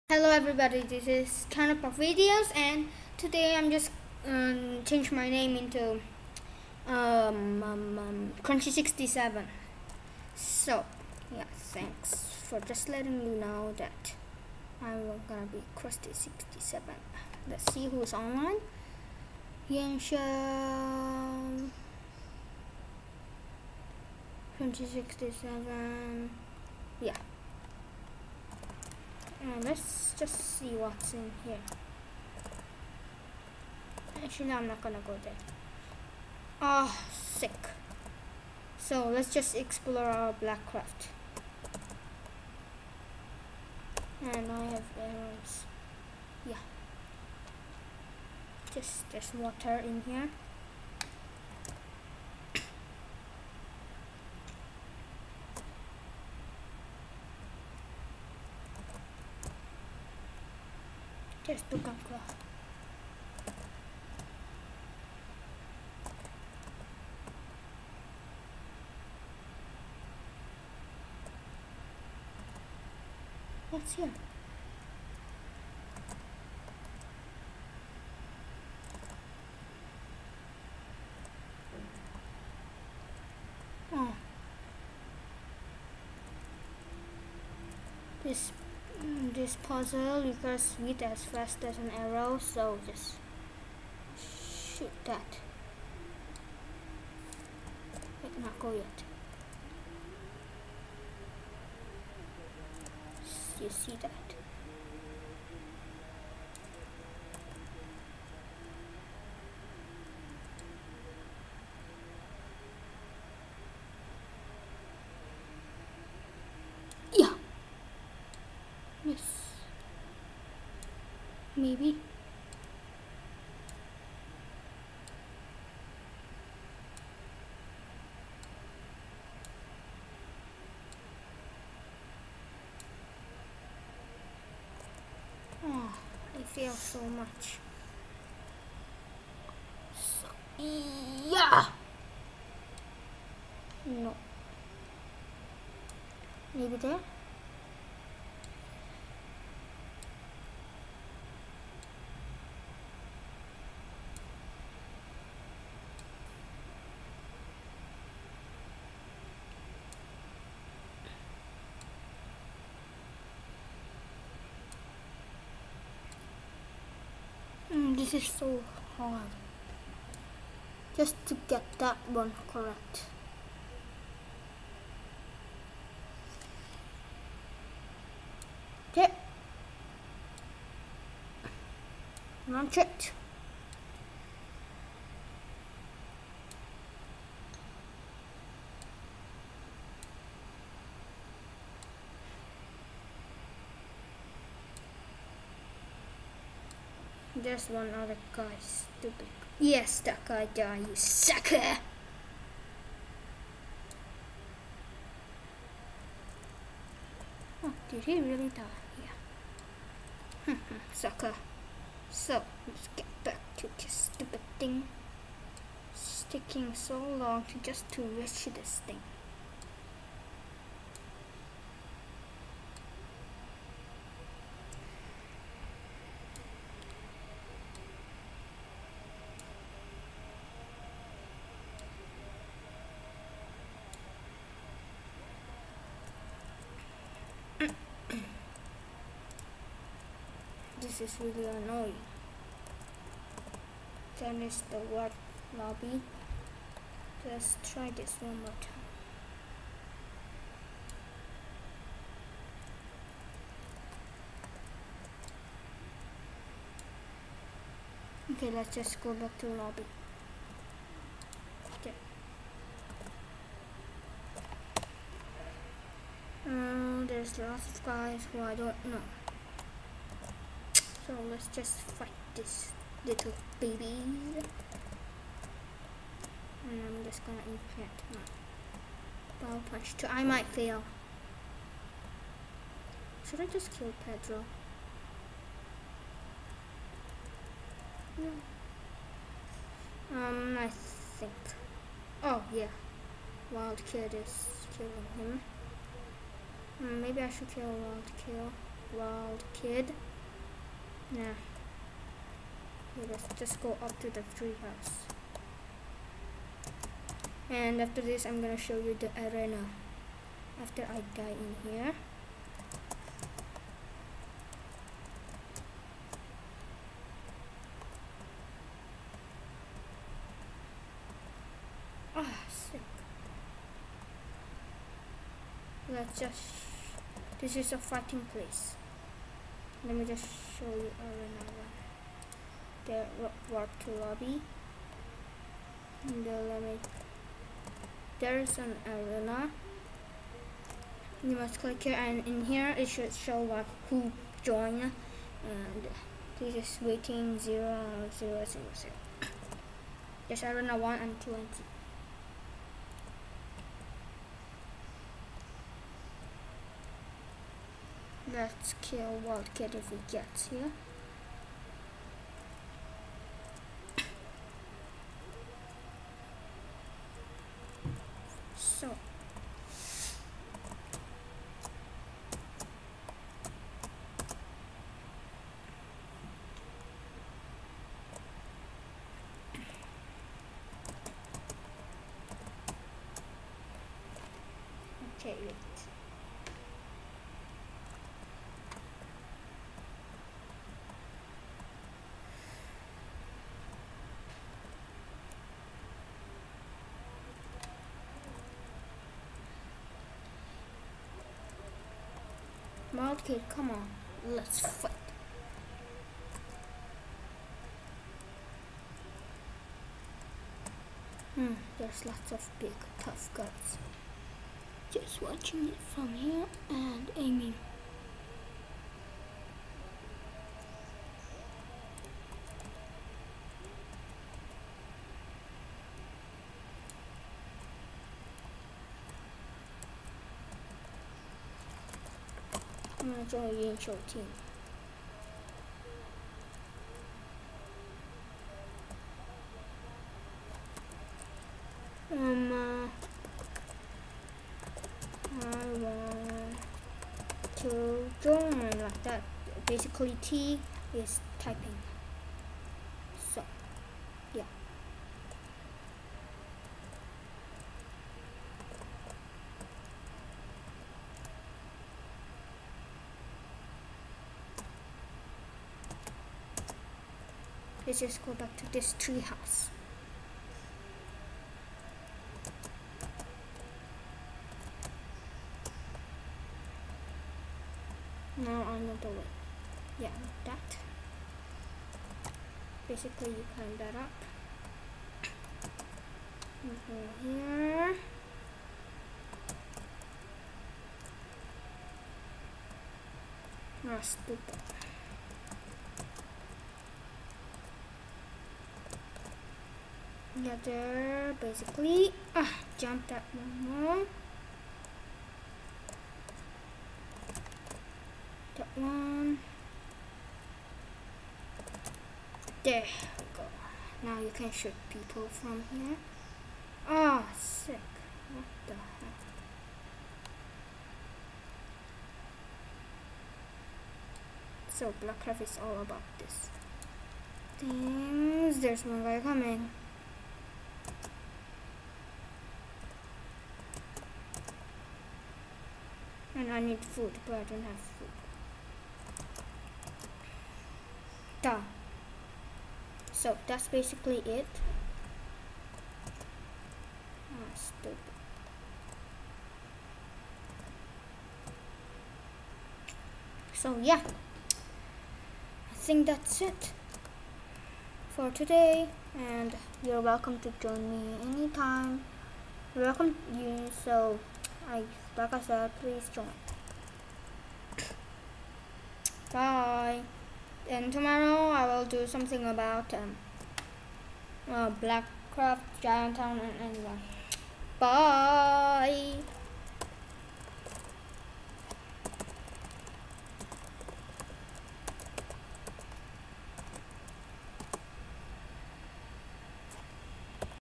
Let's Play Blackcraft With Chan Woo!
We're now going to play blackcraft server.
Server address for blackcraft: blackcraft.us